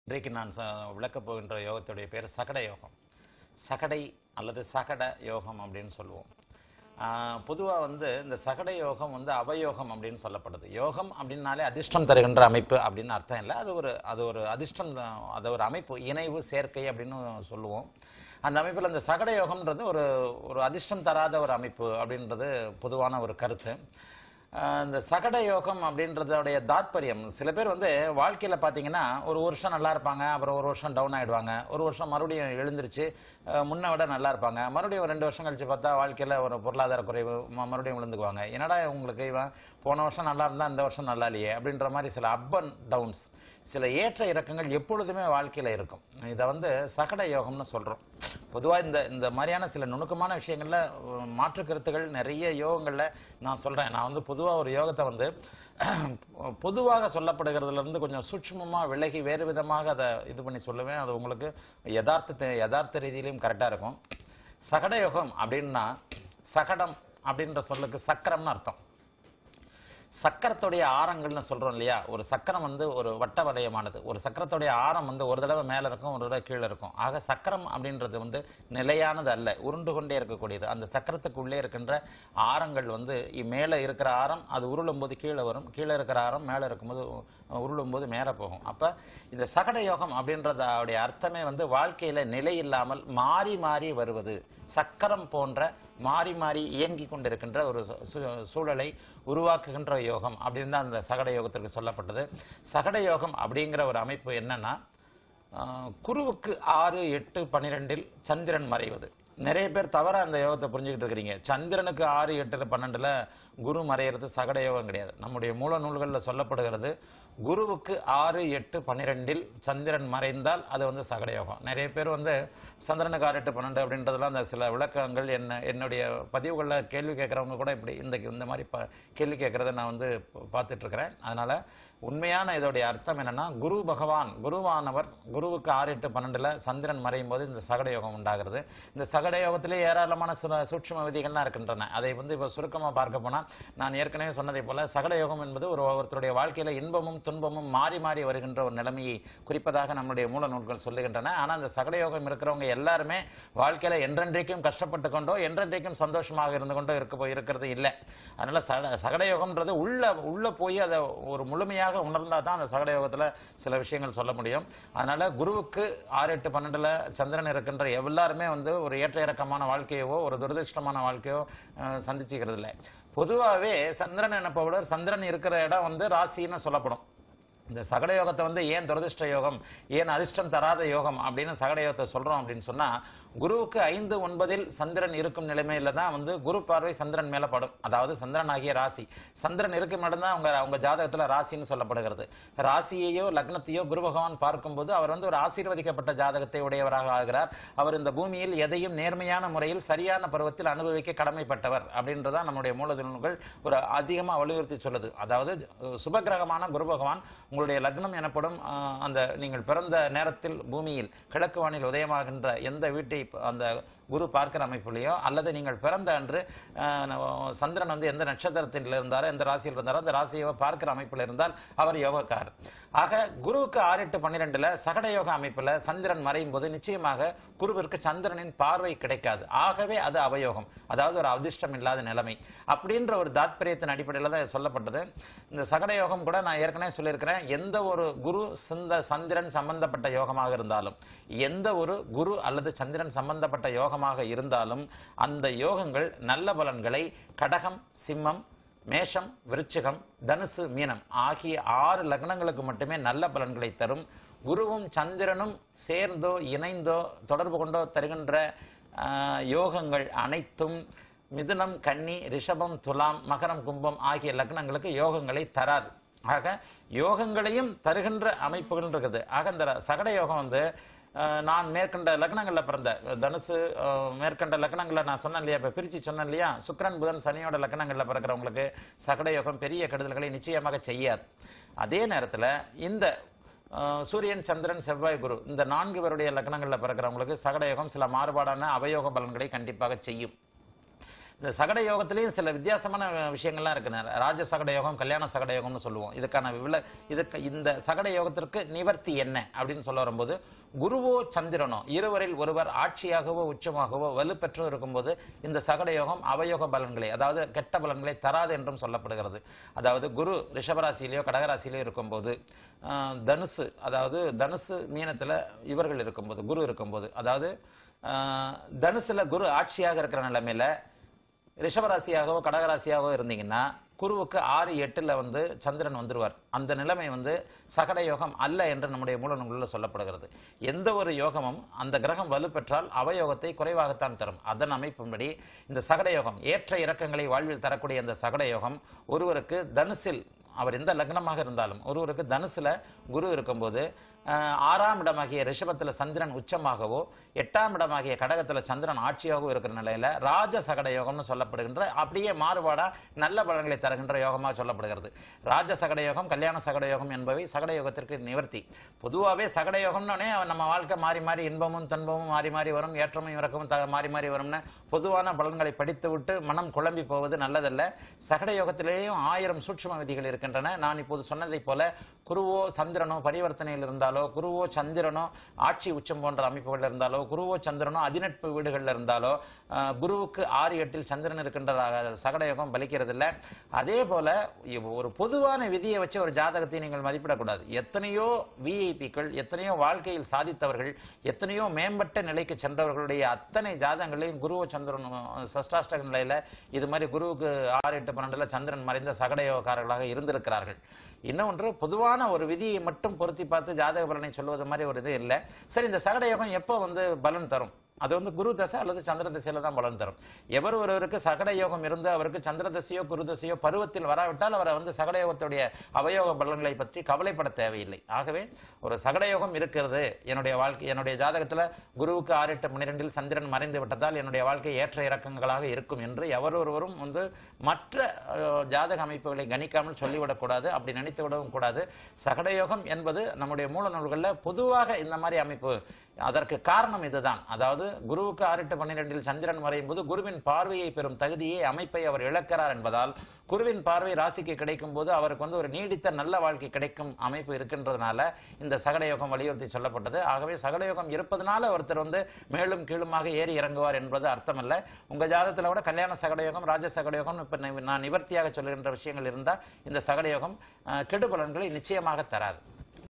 0206 - What does Sakada Yoga do? - சகட யோகம் என்ன செய்யும்? குருஜி விளக்கம்.
சகட யோகம் - SAKADA YOKAM - என்ன செய்யும்? குருஜியின் விளக்கம்... -  0139